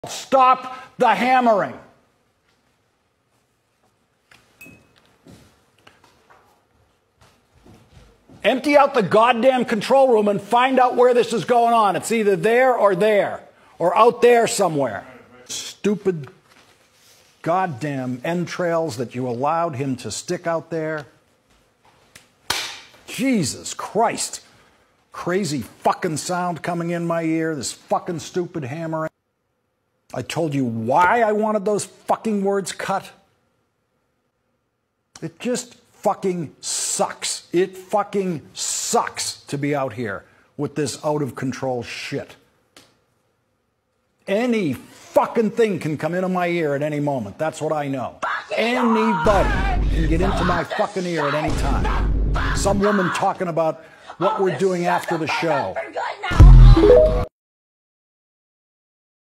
Larry O'Donnell FLIPPING OUT BEHIND THE CAMERA
This is who we get our news from people...MSNBC. Good grief lol